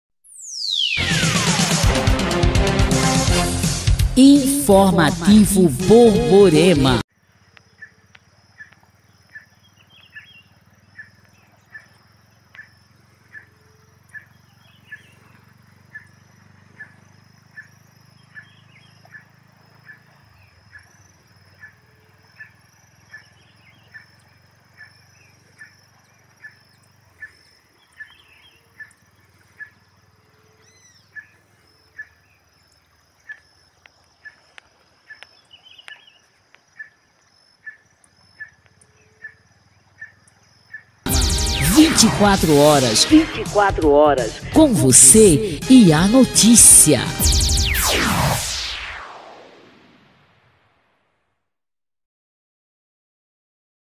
Imagens do Rio Taperoá na manhã desta terça-feira 12/03/2024, toda essa água desagua no Rio Paraíba
Imagens do Rio Taperoá na manhã desta terça-feira 12/03/2024, toda essa água desagua no Rio Paraíba que segue em direção a Boqueirão que está com mais de 43% de sua capacidade